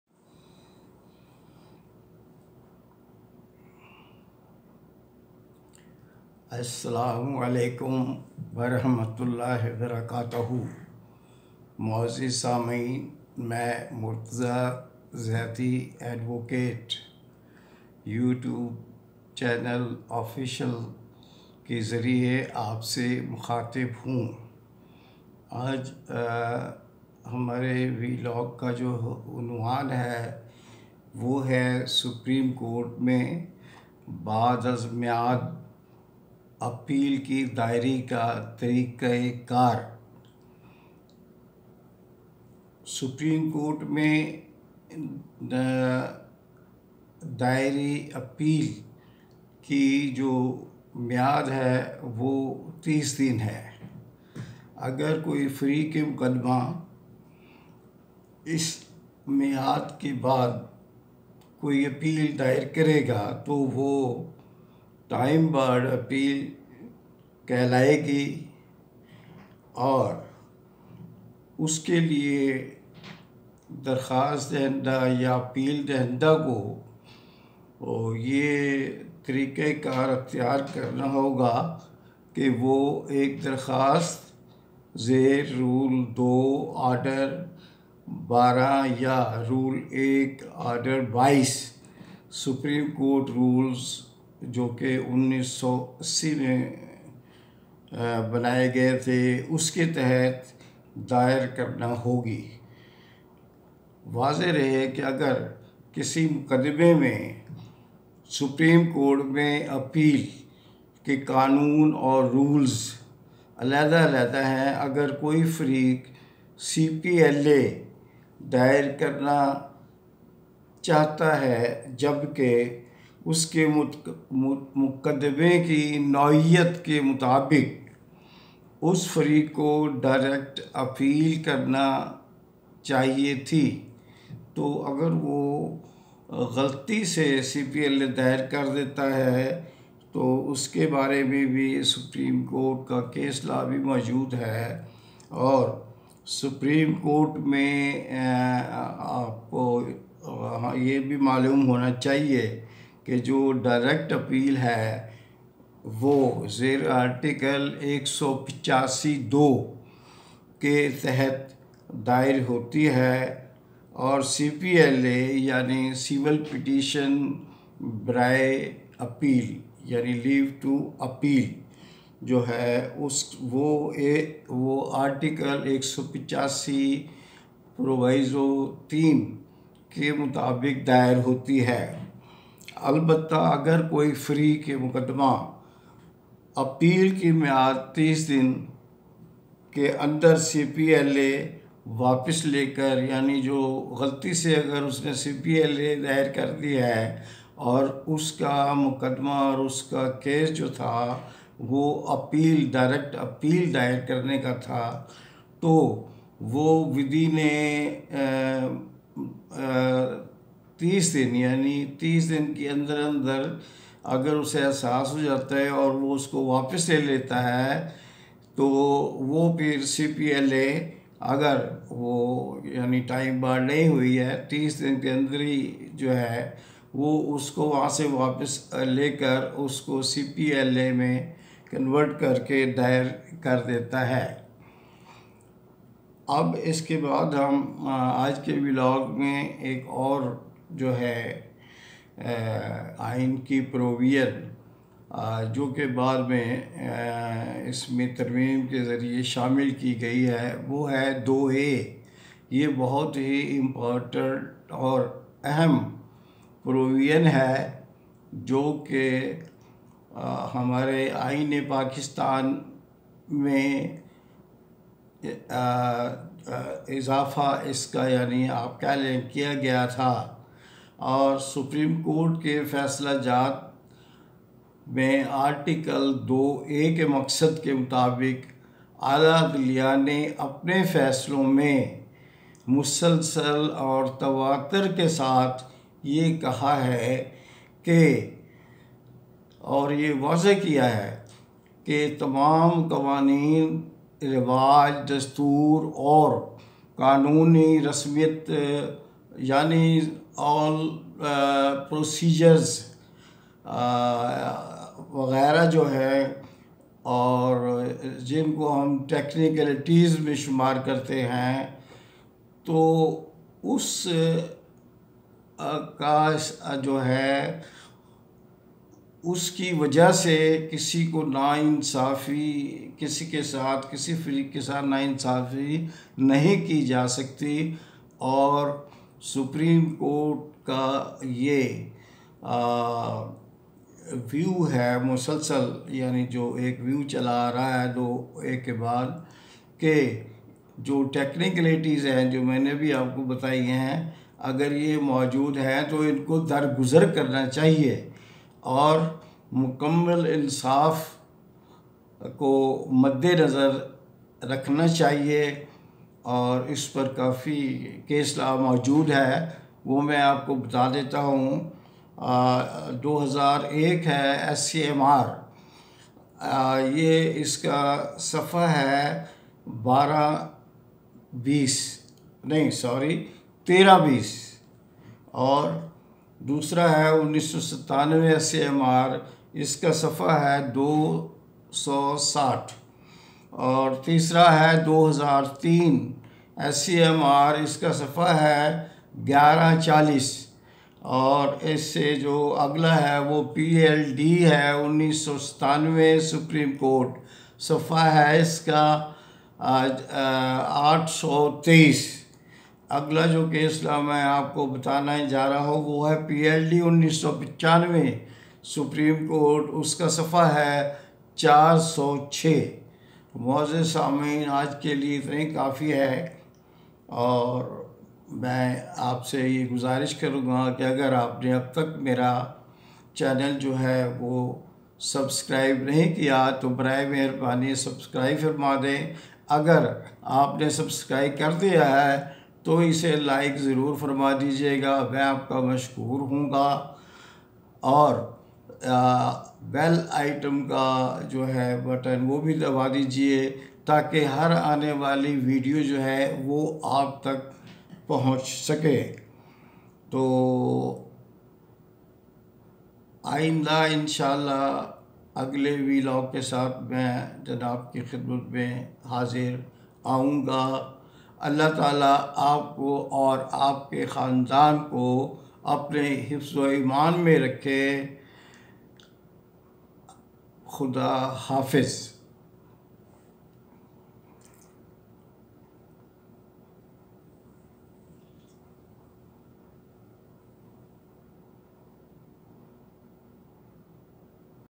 haw to get extension of time.
this video depicts remedy of  filings time barred Appeal before supreme court of pakistan u /order xii Rule 2 of supreme court  RULES 1980   By Advocate Murtaza Zaidi having 50 years standing at Bar and ex Member Executive Committee Supreme Court Bar consecutively for tow years